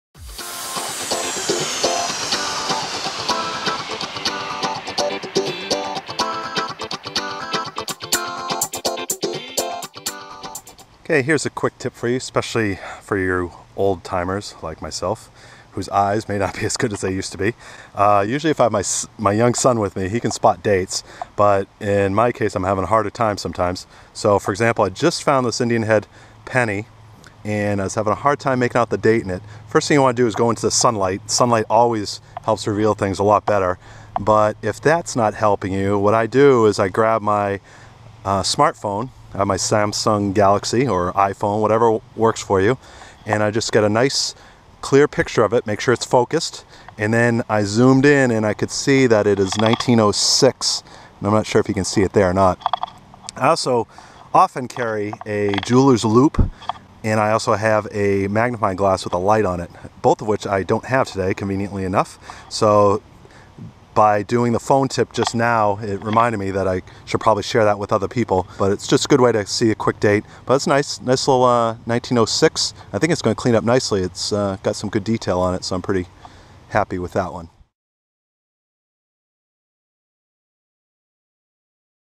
Metal detecting tip for reading dates on old coins
I use this tip more often than I should be admitting(!). Quick way to make out a tough to read date. -- More recommended reading at: metaldetectortips.com